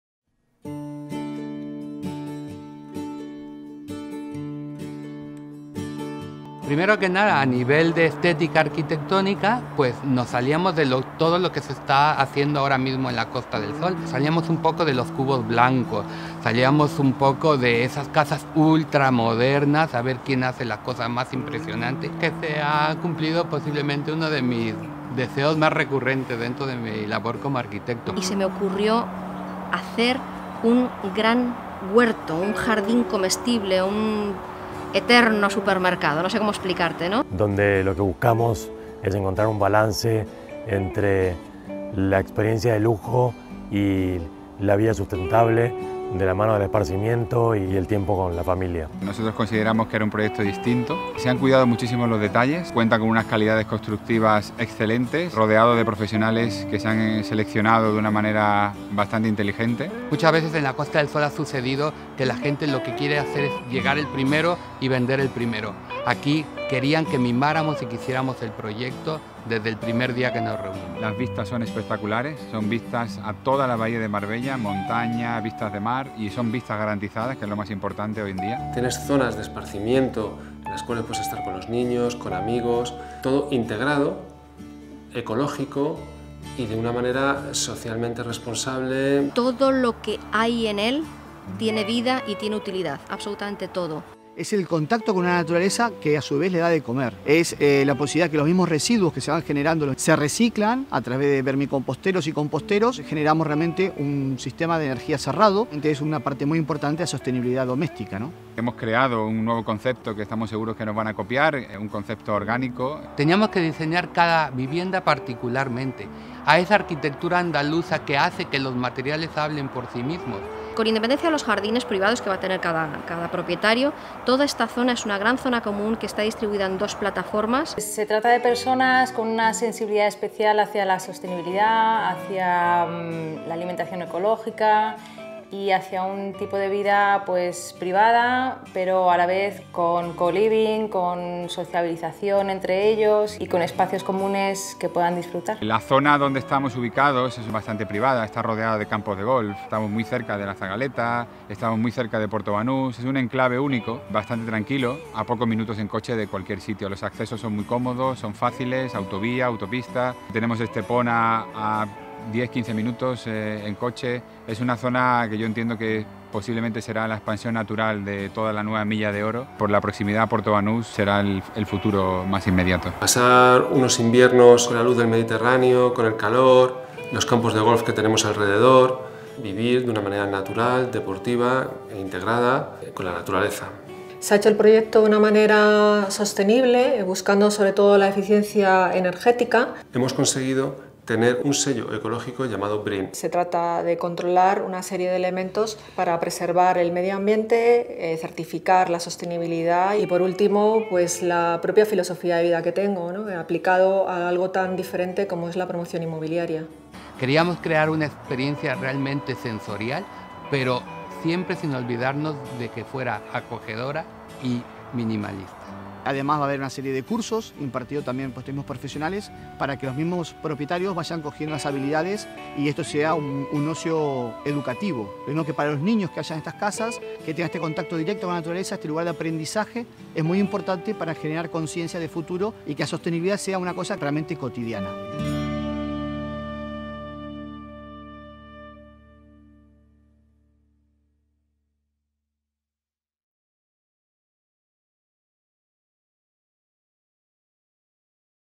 Be Lagom el Primer Proyecto Ecologicoen Costa del Sol
Disfruta de sus más de 5.000 m2 de espacios verdes formados por jardines mediterráneos con pérgolas y huertos piscinas y terrazas. Un oasis natural rodeado de bosques, árboles frutales y flores. Conecta tu esencia con la naturaleza. Estas 13 excepcionales villas diseñadas con las mejores calidades, están a cargo del prestigioso estudio de arquitectura González & Jacobson, que hacen de cada unidad una auténtica obra de arte con materiales que no esconden su esencia, aportan calidez y sirven a la arquitectura.